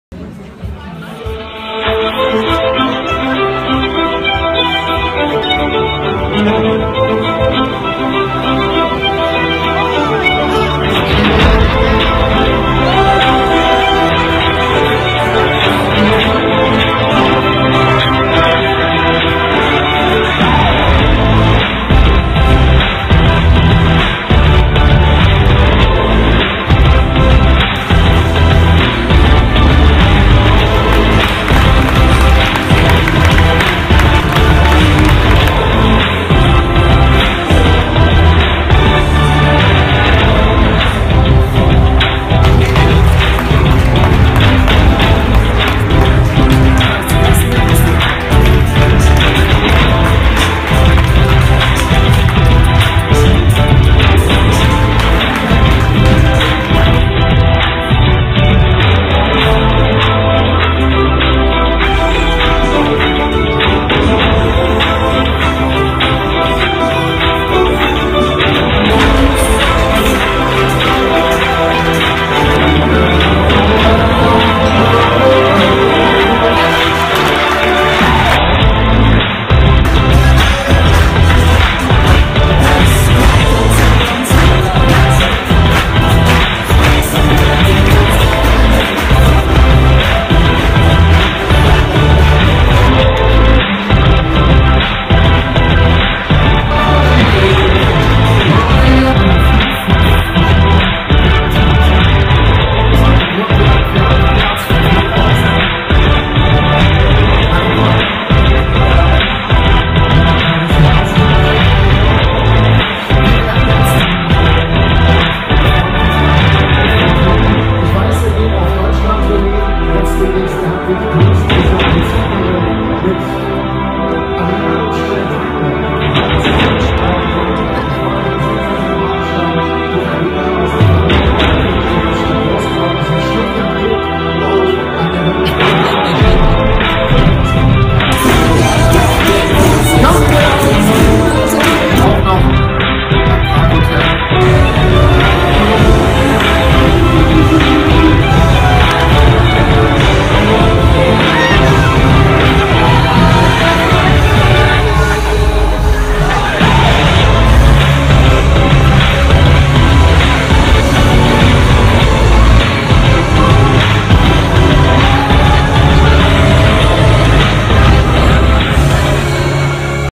Miss Tegernsee Wahl
Die Wahl zur schönsten Frau im Tegernseer Wahl. Miss Germany Soraya Kohlmann war auch da...